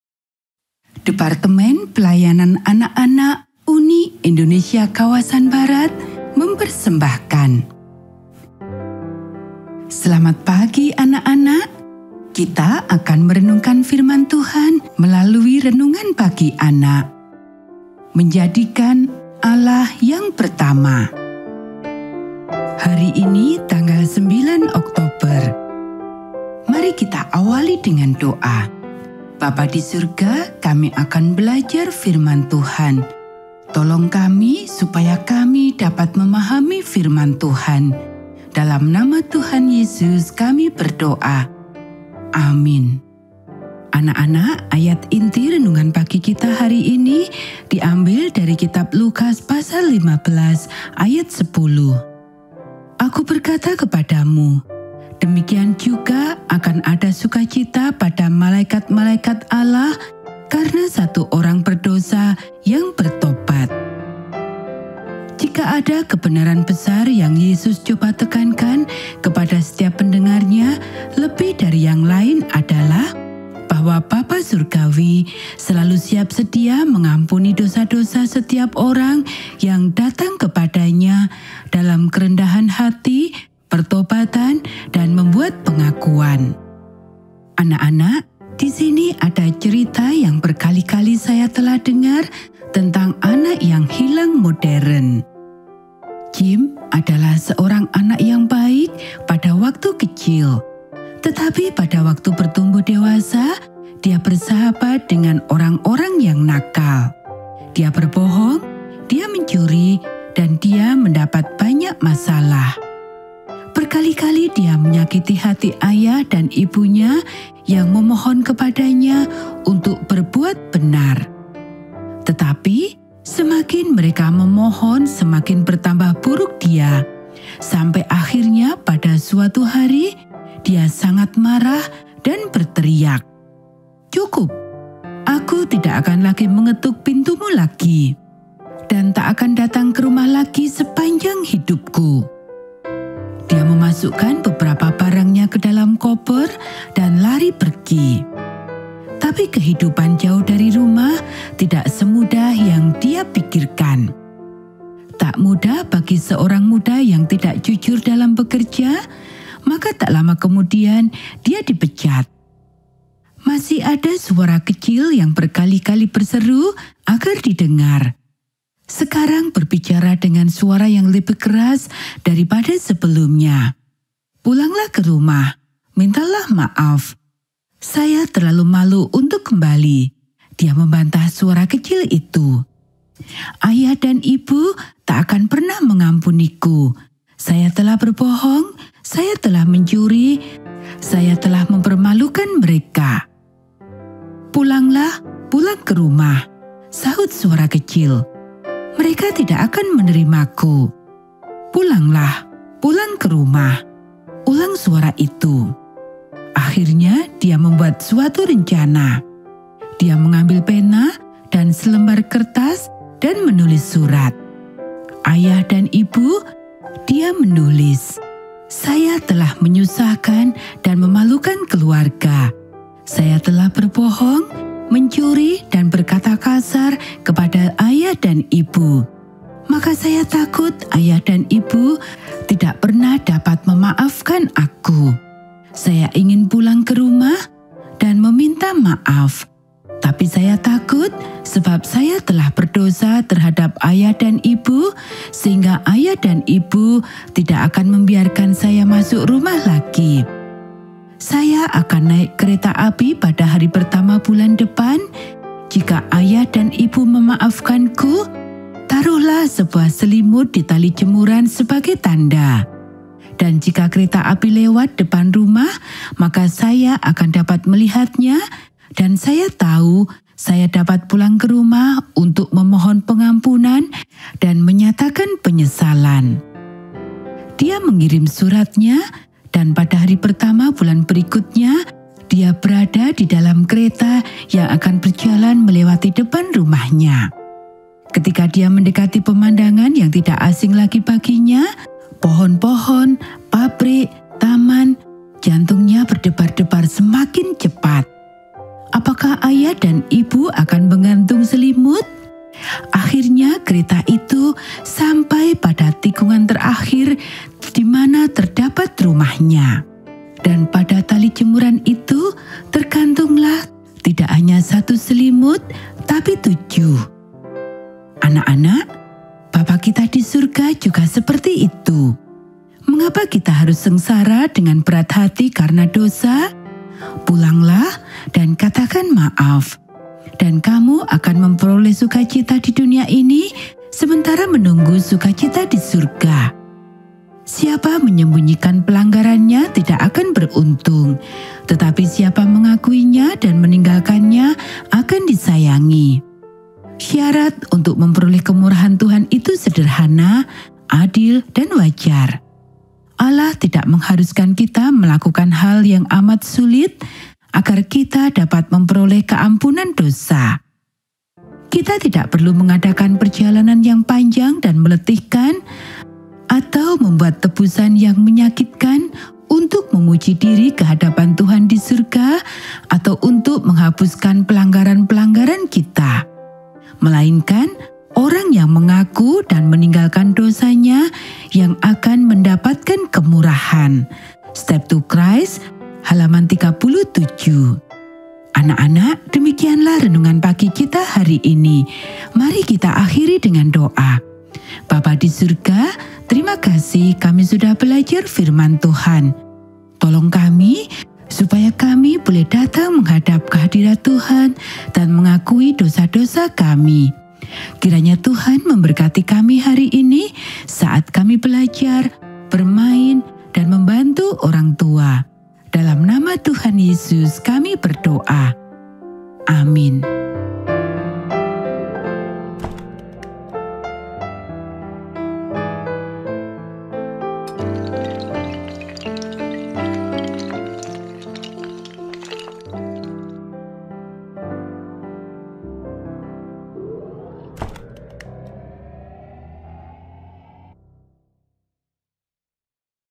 Renungan Pagi Anak 2024 | 9 Oktober
*💖"RENUNGAN PAGI ANAK 💖 BILINGUAL"*

2⃣0⃣2⃣4️⃣

*◐MENJADIKAN ALLAH YANG PERTAMA◑*

         *◦•●◉✿(9 Oktober]✿◉●•◦*

╔ ஜ۩۞۩ஜ ╗
📝 *Ayat Hafalan: Lukas 15:10*
╚ ஜ۩۞۩ஜ ╝

*– "Aku berkata kepadamu: Demikian juga akan ada sukacita pada malaikat-malaikat Allah karena satu orang berdosa yang bertobat."*

Jika ada kebenaran besar yang Yesus coba tekankan kepada setiap pendengar-Nya lebih dari yang lain adalah bahwa Bapa surgawi selalu siap sedia mengampuni dosa-dosa setiap orang yang datang kepada-Nya dalam kerendahan hati pertobatan dan membuat pengakuan. Disini ada cerita yang berkali-kali saya telah dengar tentang anak yang hilang modern

Jim adalah seorang anak yang baik pada waktu kecil, tapi pada waktu bertumbuh dewasa dia bersahabat dengan orang-orang yang nakal. Dia berbohong, dia mencuri, dan dia mendapat banyak masalah. Berkali-kali dia menyakiti hati ayah dan ibunya yang memohon kepadanya untuk berbuat benar. Tetapi semakin mereka memohon semakin bertambah buruk dia, sampai akhirnya pada suatu hari dia sangat marah dan berteriak, "Cukup! Aku tidak akan lagi mengetuk pintumu lagi, dan tak akan datang kerumah lagi sepanjang hidupku " Dia memasukkan beberapa barangnya kedalam kopor dan lari pergi.

Tapi kehidupan jauh dari rumah tidak semudah yang dia pikirkan. Tak mudah bagi seorang muda yang tidak jujur dalam bekerja, maka tak lama kemudian dia di pecat. Masih ada suara kecil yang sudah berkali-kali berseru agar di dengar, sekarang berbicara dengan suara yang lebih keras dari pada sebelumnya. "Pulanglah kerumah! Mintalah maaf!" "Saya terlalu malu untuk kembali," dia membantah suara kecil itu. "Ayah dan ibu tak akan pemah mengampuniku. Saya telah berbohong, saya telah mencuri; saya telah mempermalukan mereka." "Pulanglah! Pulang kerumah!." Sahut suara kecil. "Mereka tidak akan menerimaku " "Pulanglah! Pulang kerumah!" Ulang suara itu.

Akhimya dia membuat suatu rencana. Dia mengambil pena dan selembar kertas dan menulis surat. "Ayah, Ibu," dia menulis "Saya telah menyusahkan dan memalukan keluarga. Saya telah berbohong, mencuri dan berkata kasar kepada ayah dan ibu, maka saya takut ayah dan ibu tidak pemah dapat memaafkan ku. Saya ingin pulang kerumah dan meminta maaf, tapi saya takut sebab saya telah berdosa terhadap ayah dan ibu sehingga ayah dan ibu tak akan membiarkan saya masuk rumah lagi. Saya akan naik kereta api pada hari pertama bulan depan; jika ayah dan ibu memaafkanku, taruhlah sebuah selimut di tali jemuran sebagai tanda, dan jika kereta api lewat depan rumah maka saya akan dapat melihatnya dan saya tahu saya dapat pulang kerumah untuk memohon pengampunan dan menyatakan penyesalan".

Dia mengirimkan suratnya, dan pada hari pertama bulan berikutnya dia berada dalam kereta yang akan berjalan melewati depan rumahnya. Ketika dia mendekati pemandangan yang tak asing lagi baginya, pohon-pohon, pabrik, taman, jantungnya berdebar-debar semakin cepat, "Apakah ayah dan ibu akan menggantung selimut..?" Akhimya kereta itu sampai pada tikungan terakhir dimana terdapat rumahnya! Dan pada tali jemuran itu tergantunglah, tidak hanya satu selimut tapi tujuh.

Bapa kita disurga juga seperti itu. Mengapa kita harus sengsara dengan hati berat karena dosa? Pulanglah dan katakan maaf Dan kamu akan memperoleh suka cita di dunia ini sementara menunggu suka cita di surga. "Siapa menyembunyikan pelanggarannya tidak akan beruntung, tetapi siapa mengakuinya dan meninggalkannya akan disayangi" Syarat untuk memperoleh kemurahan Tuhan itu sederhana, adil dan wajar. Allah tidak mengharuskan kita melakukan hal yang amat sulit agar kita dapat memperoleh keampunan dosa. Kita tidak perlu mengadakan perjalanan yang panjang dan meletihkan, atau membuat tebusan yang menyakitkan, untuk memuji diri kehadapan Tuhan di surga atau untuk menghapuskan pelanggaran-pelanggaran kita; melainkan orang yang mengaku dan meninggalkan dosanya yang akan mendapat kemurahan.." Steps to Christ, p.37.